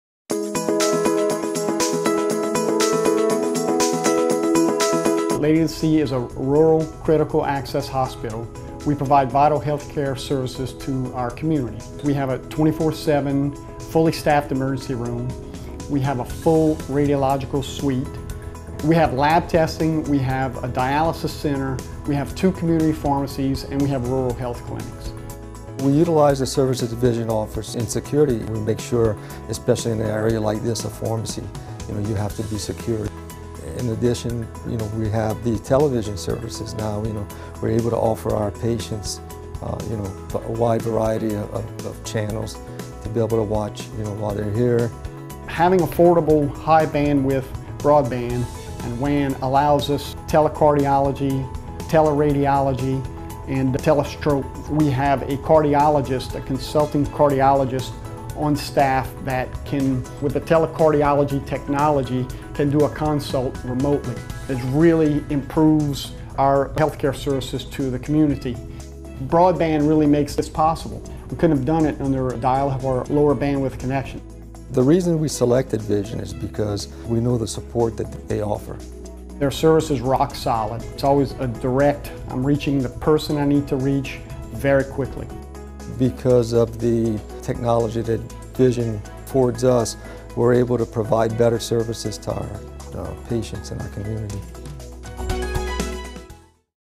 Lady of the Sea + Vision Communications
Lady of the Sea explains how Vision Communications helps to power their healthcare services for the local community through security, television and broadband.